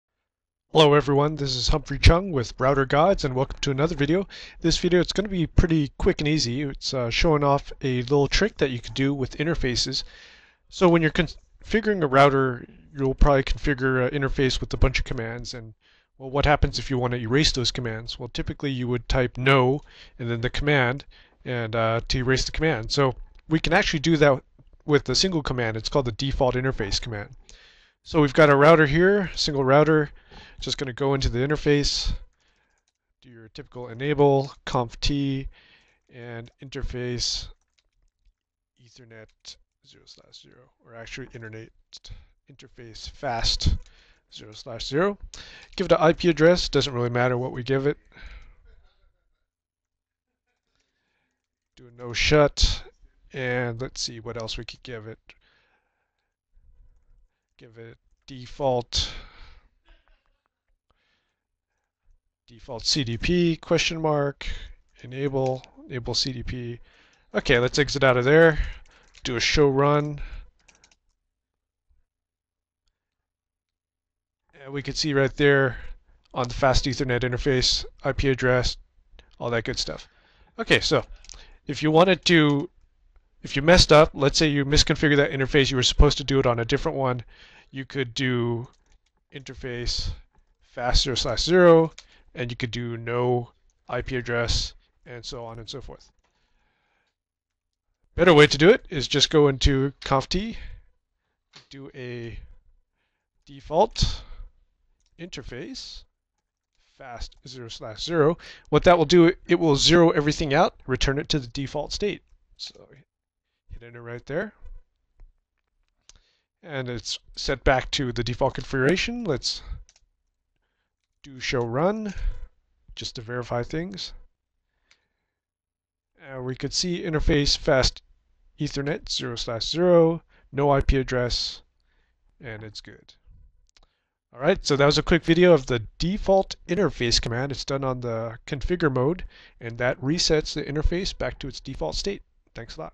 Default Interface command in Cisco IOS
Easily reset your interface back to factory default with this command